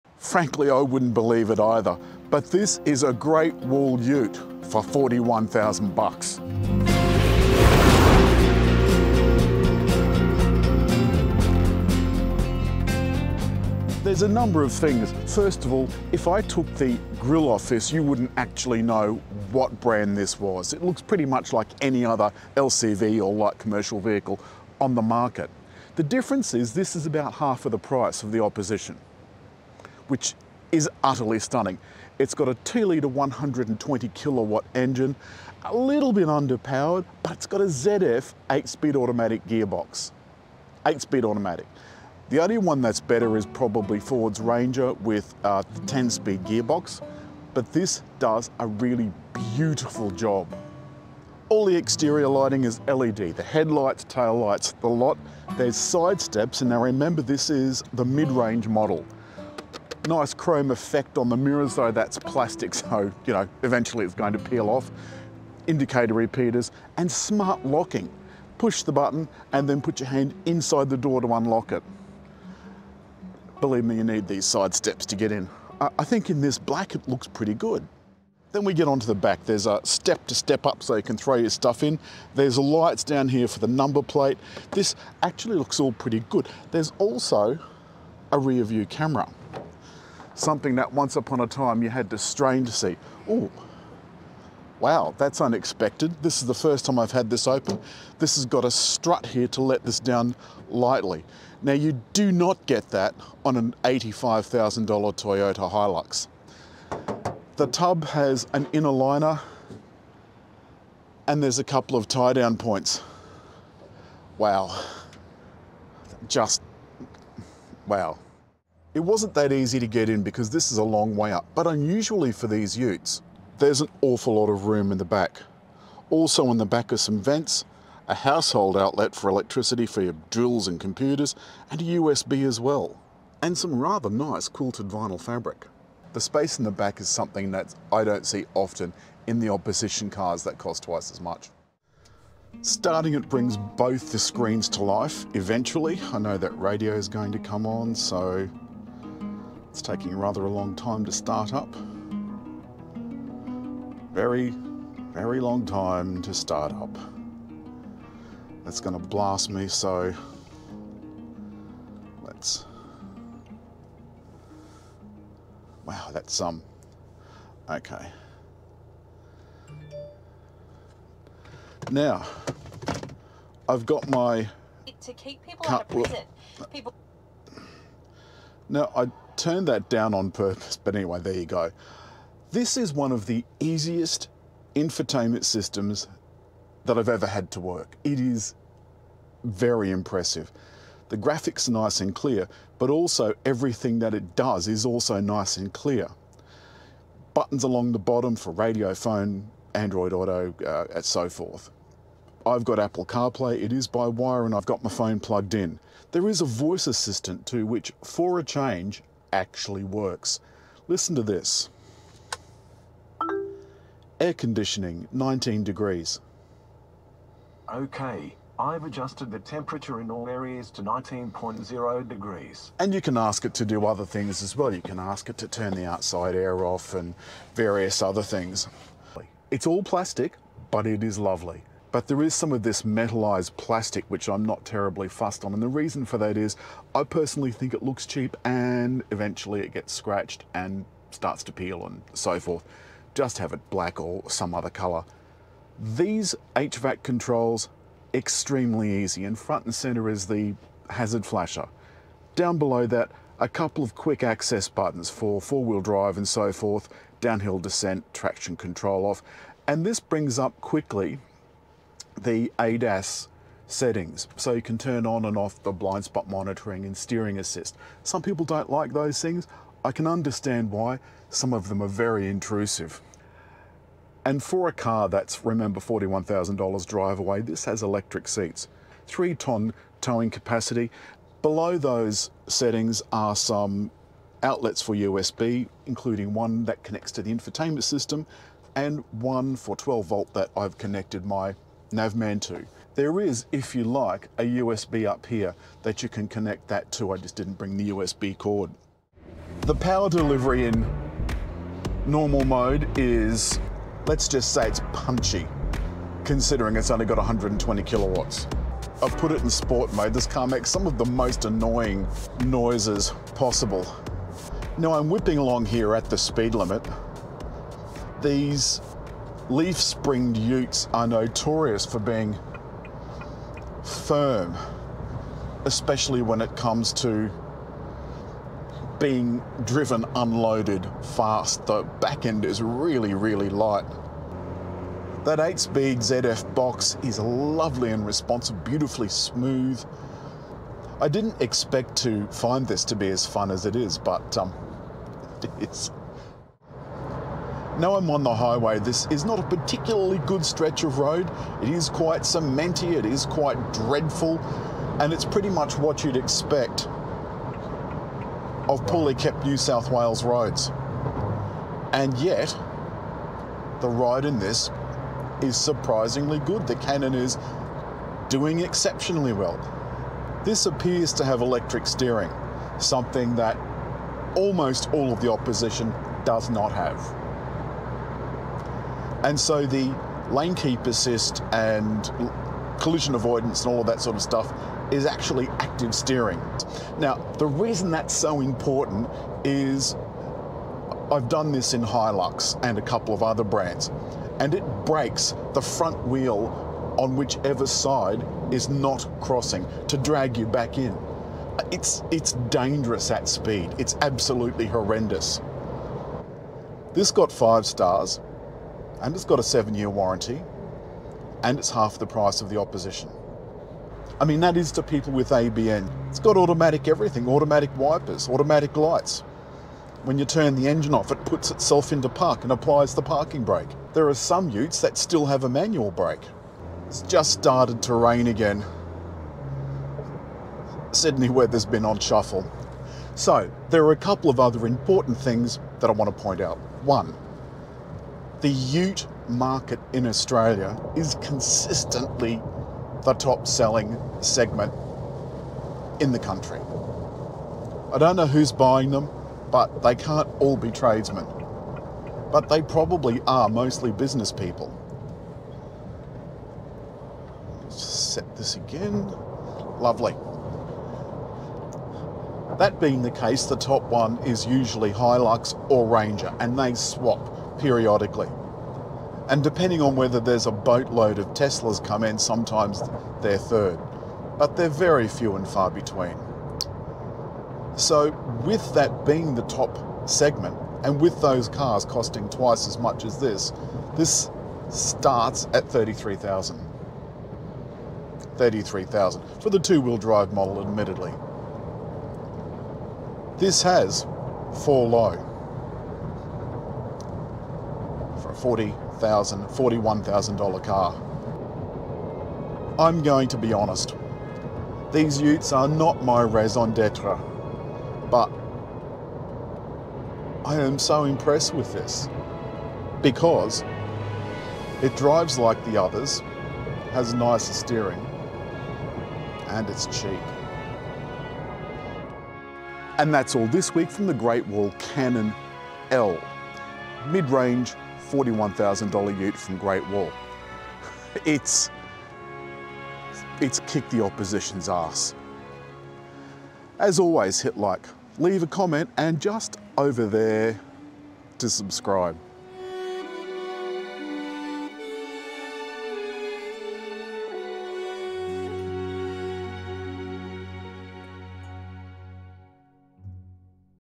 Is GWM Cannon Pickup Ute FABULOUS Value - FULL Review
The 4-range Cannon line-up is reasonably well priced considering the gear they’ve managed to shoehorn in.

 Full Review and Brochure HERE: Is GWM Cannon Pickup a Good Value Good Drive REVIEW

The base model is 2WD, and all the rest had AWD of 4-low range and a bunch of off-roading accoutrements.
The outside is better than expected, and the quality appears to be at least as good as its opposition
Inside:
Only the Vanta has luscious leather, and an inny-outie steering wheel. Not being able to adjust for reach is a pain, and makes the driving position less comfy than it should be.
Everything is plastic, and the metalised surround on the climate and off-road controls is a pet hate of mine. It always looks nasty, and eventually peels. It is a mere trifle in a cabin that genuinely feels posh. The bottom 3 models have plastic everywhere, including the seats. While the top 2 models have seat heating and auto climate control, all Cannons have rear vents.
The rear seat also has a nifty household socket, as well as a USB outlet. The biggest ticket item back there is bags of space. Even the tallest apprentice can stow knees without giving the boss a bump in the back.
The sound system is acceptable, with a 9” screen  and Apple CarPlay/Android Auto by USB. Vanta gets extra speakers.
The infotainment menu has a bunch of levels with nothing further than a couple of clicks away. It is a nicely laid out system, and as usual, includes the safety and driver aid settings. They can be fettled as the driver wishes, or turned off completely.
Only Vanta gets a Qi charger too, sadly absent from our L.